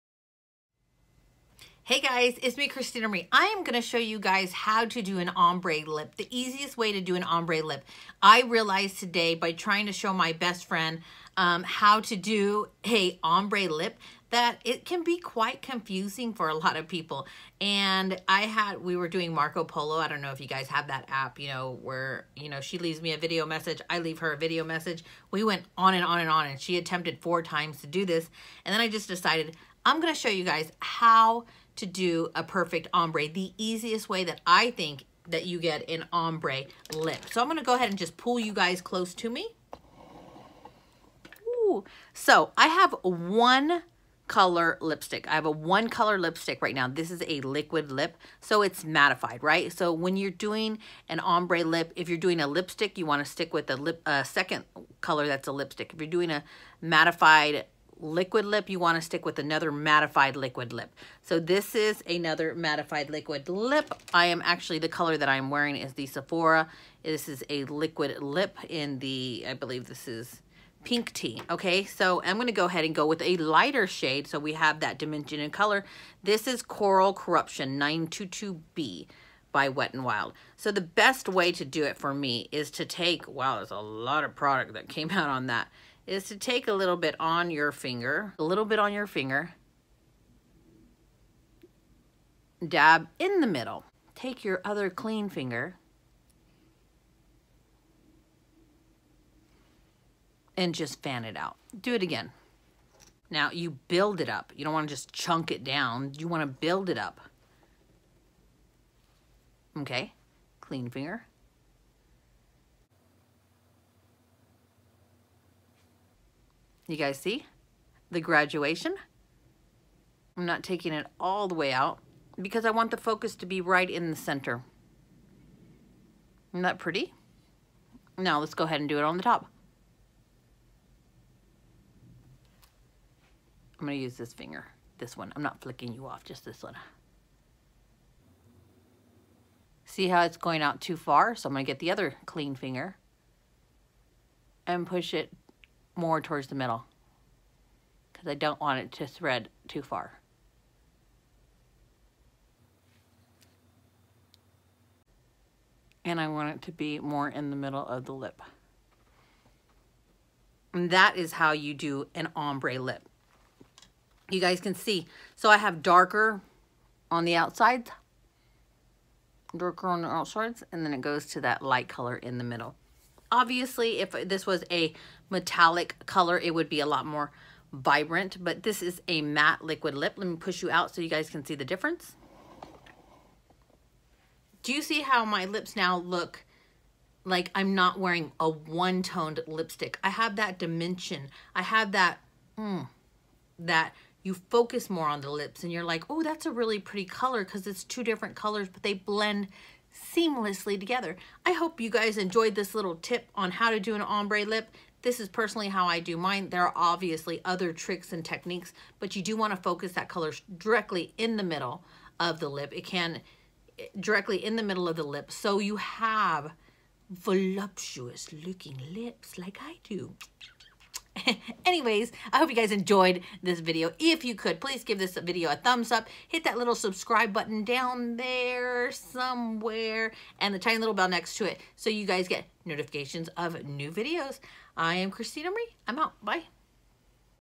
EASIEST OMBRE LIPSTICK STEP BY STEP FOR BEGINNERS
This video shows beginners how to apply an ombre lipstick look. Its super simple and easy to do.